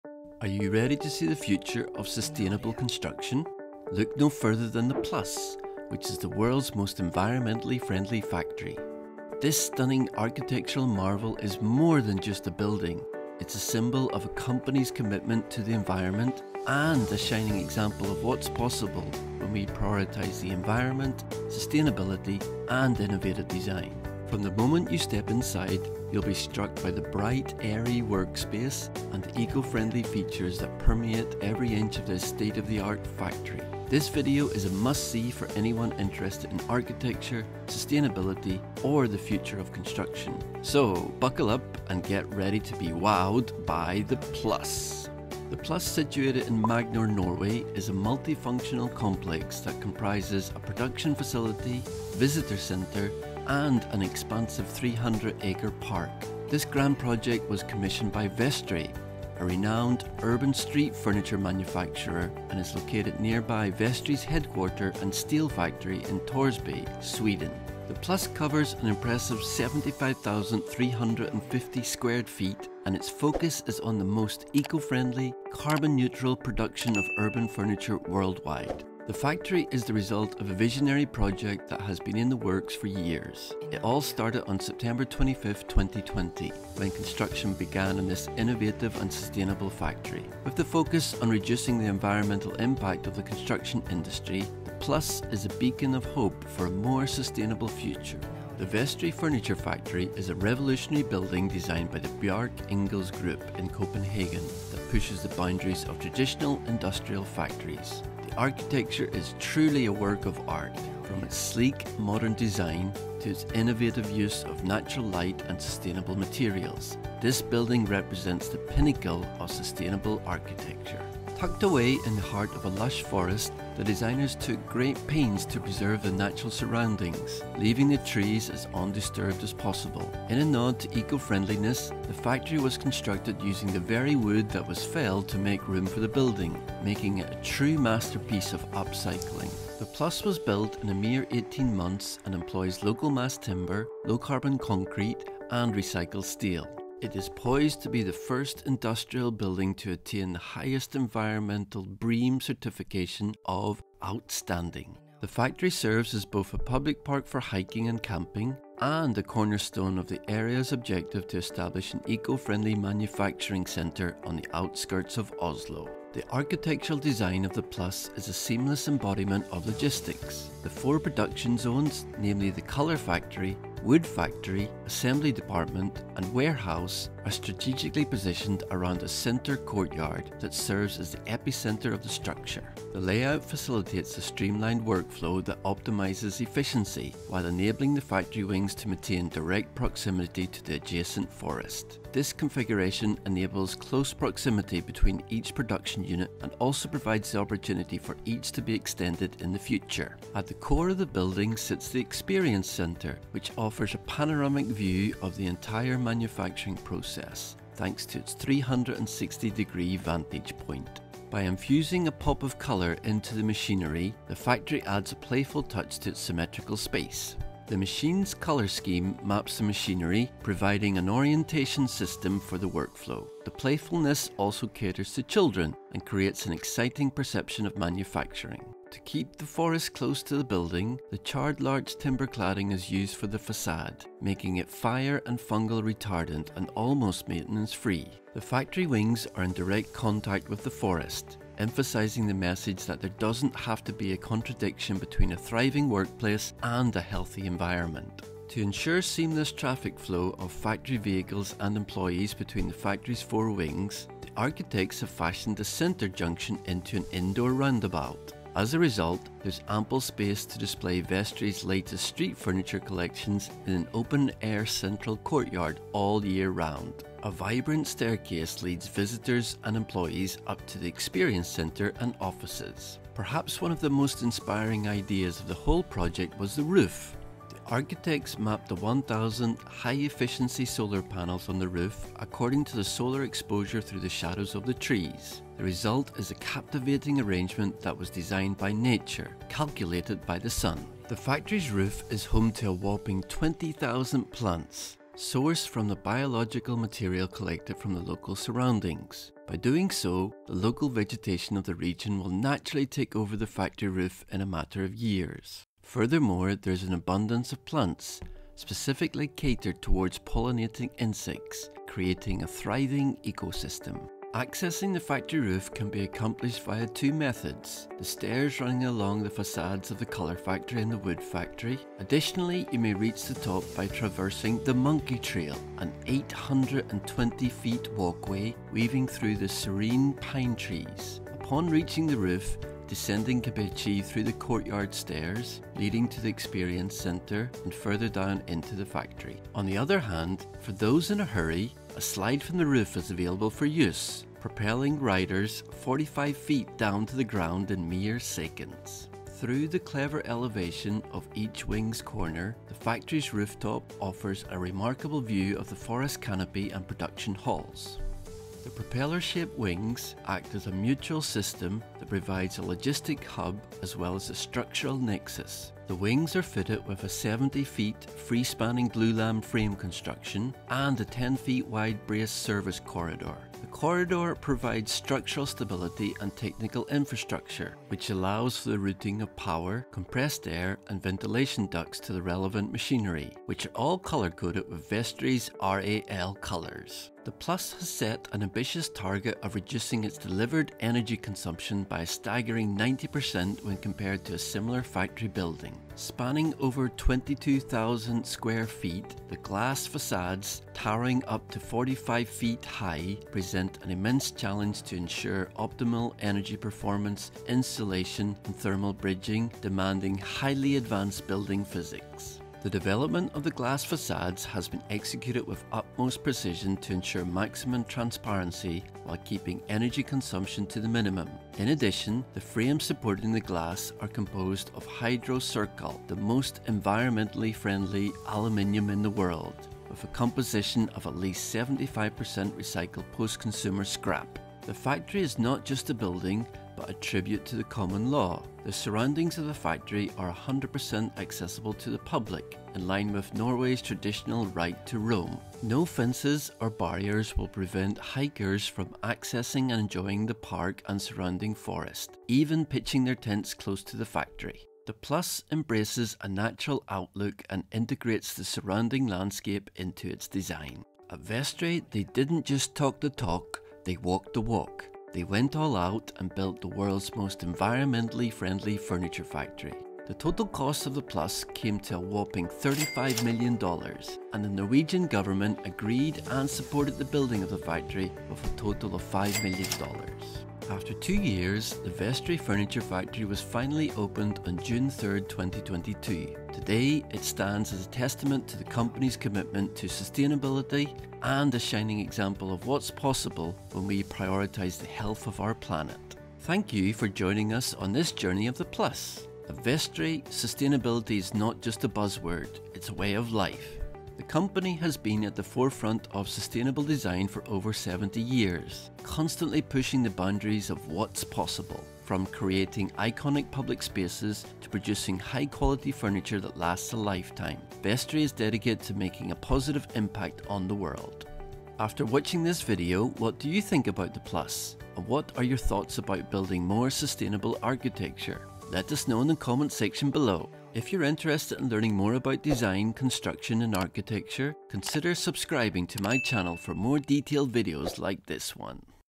The Factory Hidden in a Forest: The PLUS
Step inside the world of sustainable furniture manufacturing with Vestre's innovative factory - The Plus. Located in Magnor, Norway, this groundbreaking facility is setting a new standard for eco-friendly and socially responsible design. In this video, we'll take you on a behind-the-scenes tour of The Plus, showing you how Vestre is using advanced technology, renewable energy, and sustainable materials to create beautiful, functional, and ethical furniture.